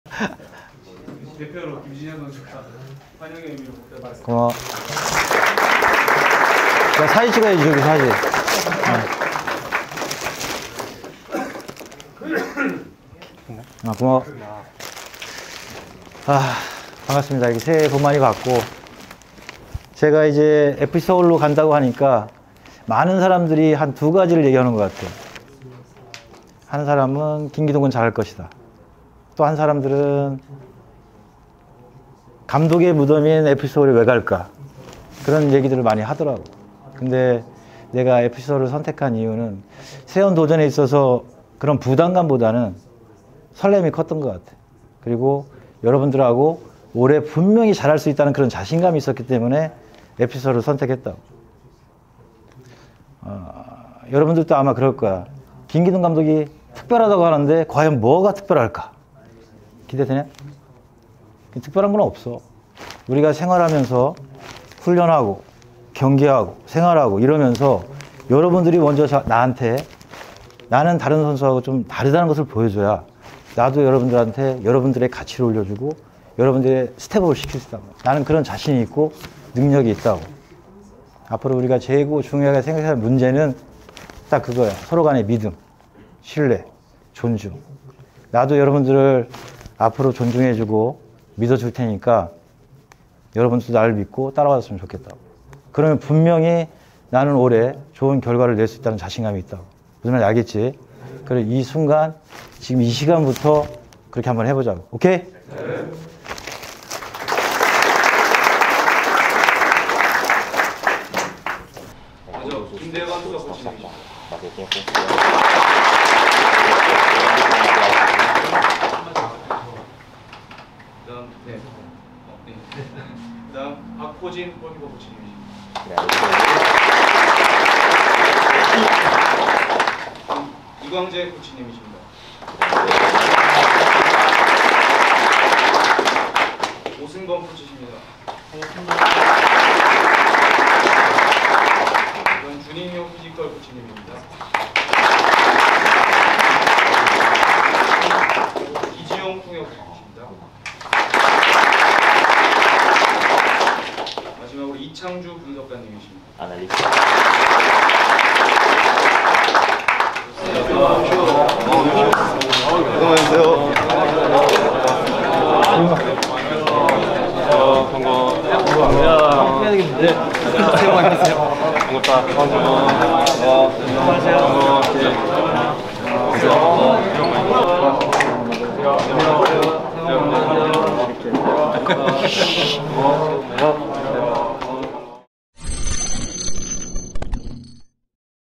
FC서울 김기동 감독과 선수단 첫 미팅
FC서울에 새롭게 부임한 김기동 감독이
오늘 GS챔피언스파크에서 선수단과
첫 만남을 진행했습니다.